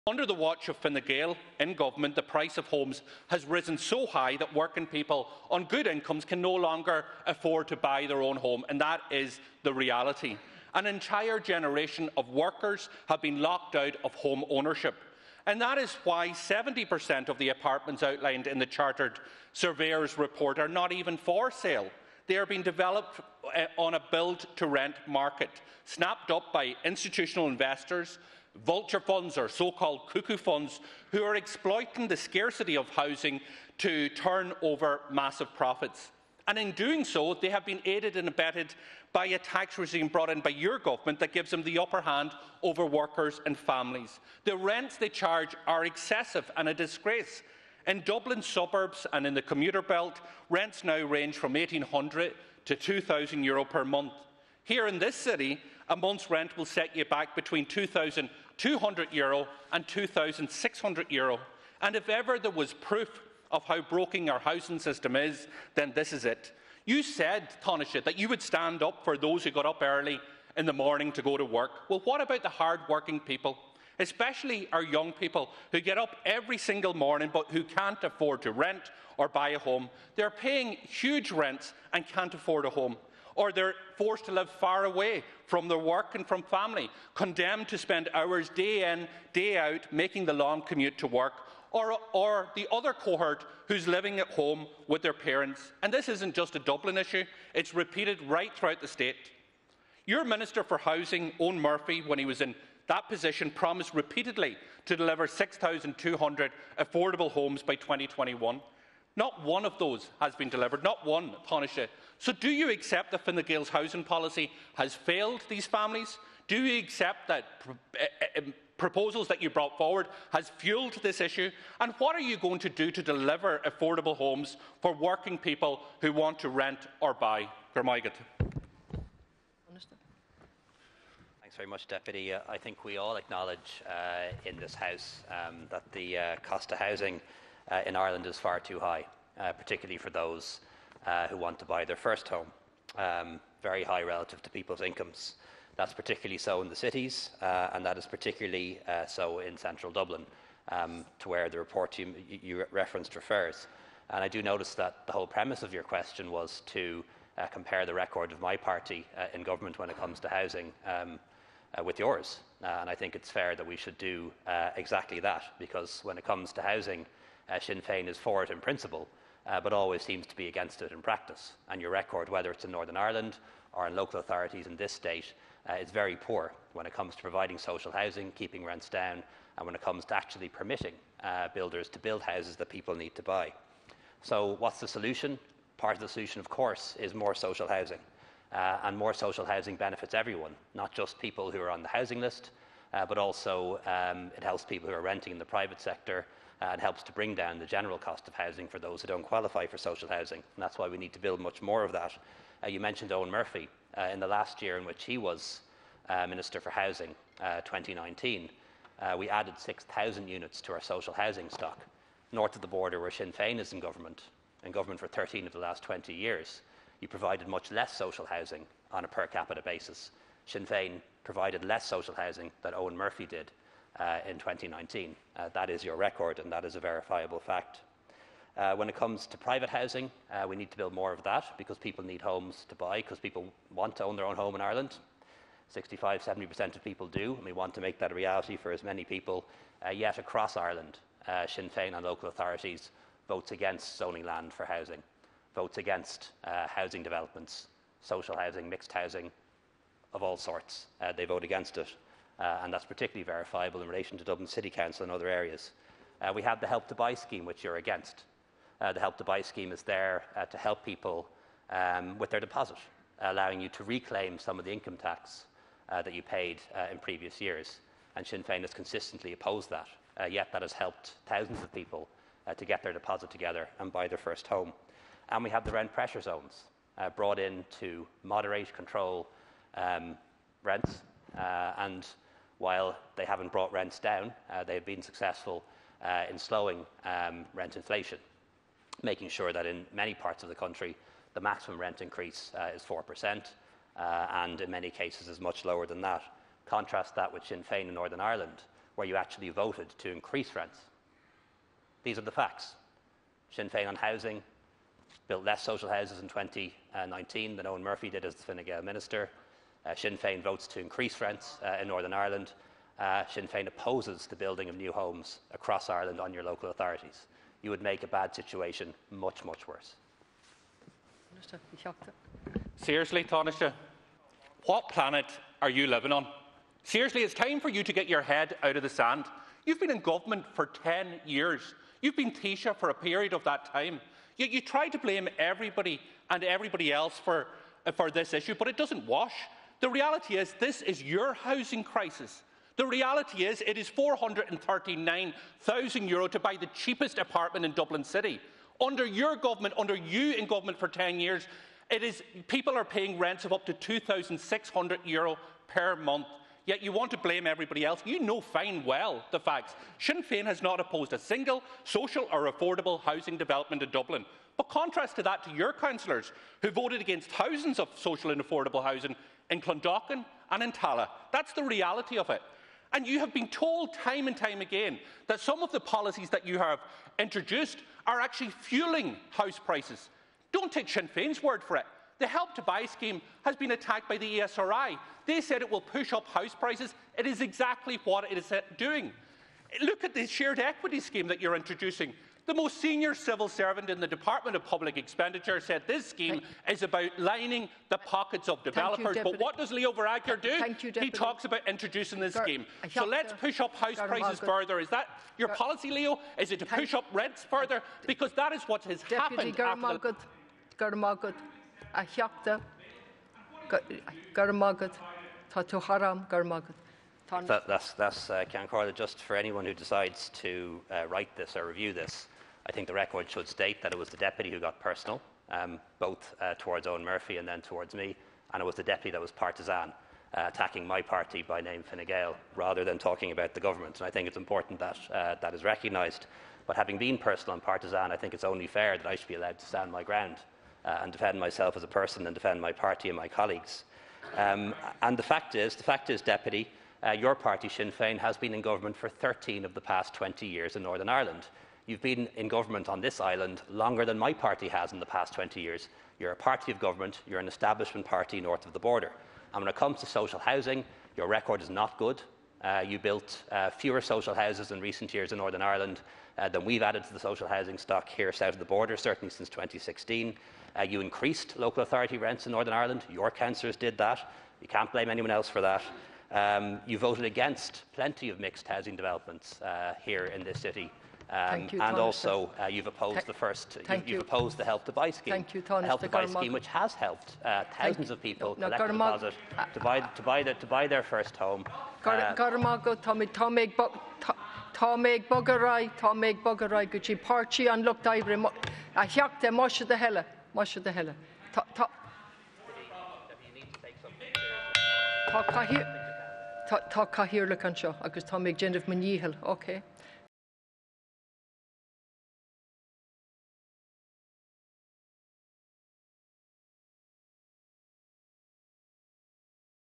"What planet are you living on?" Pearse Doherty challenges Leo Varadkar over house prices
Sinn Féin's Pearse Doherty criticized the government over house prices during Leaders' Questions.

However, the Tánaiste pointed to Sinn Fein's record in Northern Ireland.

"You built fewer social houses in recent years in Northern Ireland than we've added to the stock here, south of the border, certainly since 2016."

"Sinn Féin has not opposed a single social or affordable housing development in Dublin," Pearse Doherty responded.

"But in contrast to that, your councillors who voted against thousands of social and affordable housing in Clondalkin and in Tallaght."

Leo Varadkar then asked for it to be noted that "it was the deputy that got personal."

Video: Oireachtas TV